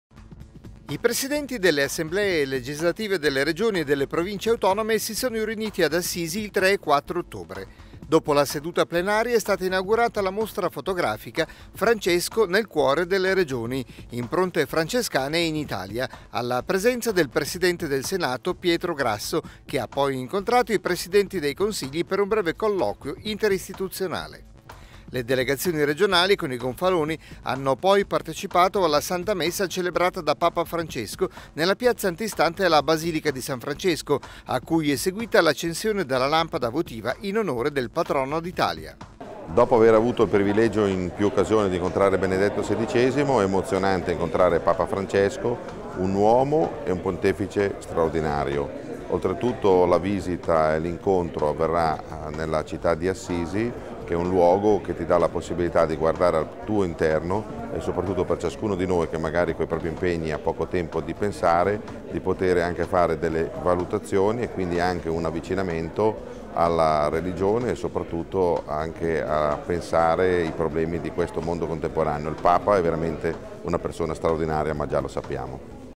I Consigli regionali ad Assisi
I presidenti delle Assemblee legislative delle Regioni e delle Province autonome si sono riuniti ad Assisi il 3 e 4 ottobre. Dopo la seduta plenaria, è stata inaugurata la mostra fotografica "Francesco nel cuore delle Regioni, impronte francescane in Italia", alla presenza del presidente del Senato, Pietro Grasso, che ha incontrato i presidenti dei Consigli per un breve colloquio interistituzionale. Le delegazioni regionali, con i gonfaloni, hanno poi partecipato alla Santa Messa celebrata da Papa Francesco nella piazza antistante la Basilica di S. Francesco, a cui è seguita l'accensione della lampada votiva in onore del Patrono d'Italia.